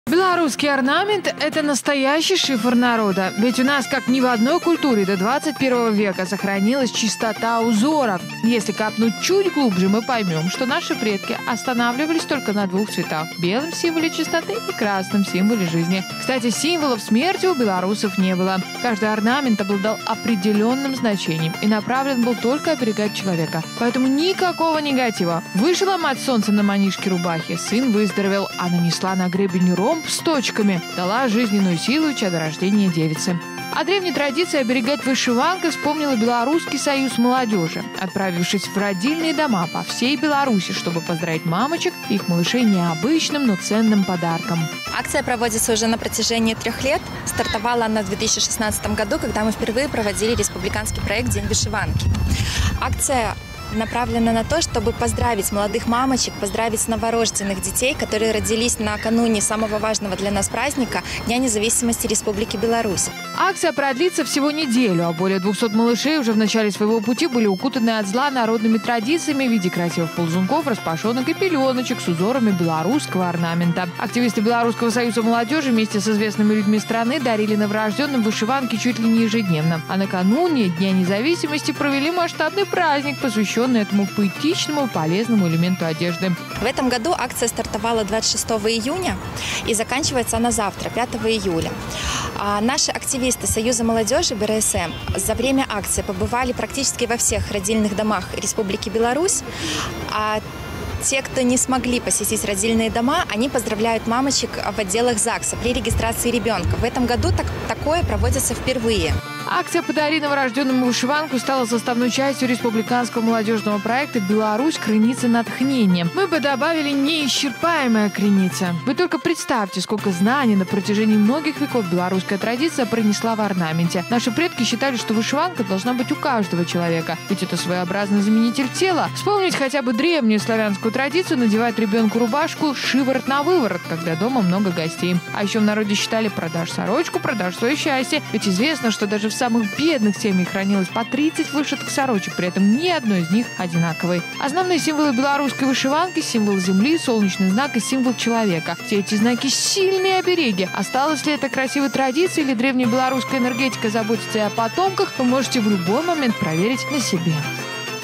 Вышиванки для новорожденных
Белорусский орнамент  - это настоящий шифр народа. Ведь у нас как ни в одной культуре до 20 века сохранилась чистота узоров. И если копнуть чуть глубже мы поймем, что наши предки останавливались только на 2 цветах, белом-символе чистоты, и красном символе жизни. Кстати, символов смерти у белорусов не было. Каждый орнамент обладал определенным значение и направлен был только оберегать человека. Поэтому никакого негатива. Вышила мать солнце на манишке рубахи — сын выздоровел, а нанесла на гребень ромб с точками — дала жизненную силу и чадорождение девице. О древней традиции оберегать вышиванкой, вспомнил и белорусский союз молодежи. Отправившись в родильные дома по всей Беларуси, чтобы поздравить мамочек и их малышей необычным, но ценным подарком.